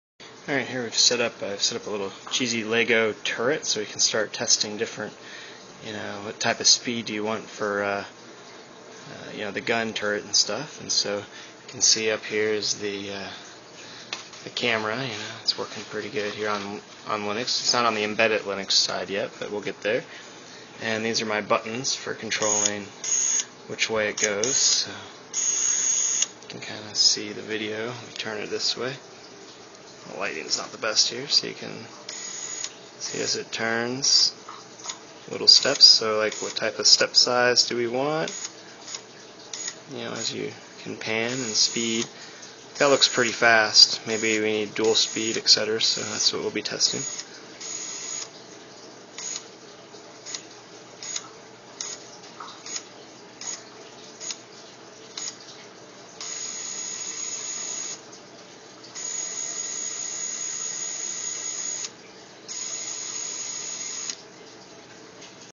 Mech Warfare Turret testing
Here is a logitech C310 camera strapped to some lego mindstorms to test out turret requirements.  How fast?  How slow?  That is the question to be solved.